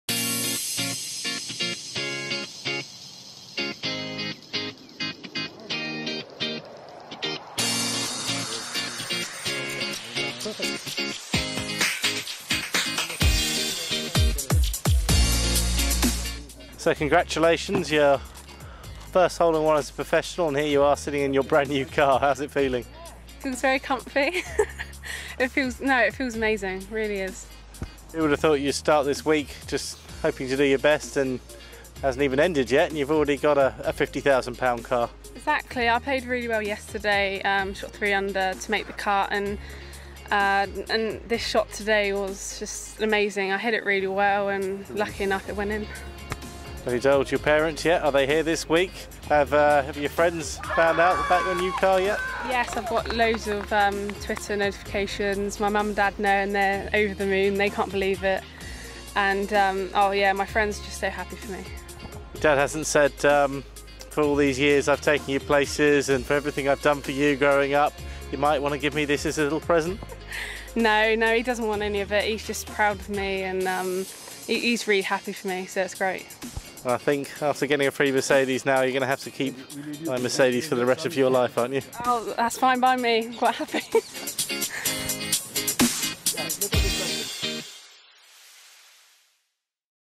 Georgia Hall Wins a Mercedes with a Hole in One!
Georgia Hall receiving the key for Mercedes Benz from Karl Sandesjo and Malcolm Cooper of Gargash Enterprises-Mercedes Benz as Mohamed Juma Buamaim, vice chairman and CEO of golf in DUBAi, looks on at the 15th hole where Hall scored a hole in one with a four iron

England’s Georgia Hall produced the shot of the day when she aced the 167-yard par-3 15th hole using a 4-iron in the third round of the Omega Dubai Ladies Masters on the Majlis Course at Emirates Golf Club.

The 18-year-old from Bournemouth in Dorset won a gleaming Mercedes Benz C 200 worth around €60,000 and will now have to buy herself some driving lessons as she can’t yet drive.

This was Georgia’s second hole in one as she aced the 17th at Machynys Peninsula Golf Club in South Wales to win the 2013 Ladies British Open Amateur Championship.

It was also the same hole where South African Connie Chen made a hole in one during the Omega Dubai Ladies Masters in 2013, for which she won a Mercedes Benz convertible.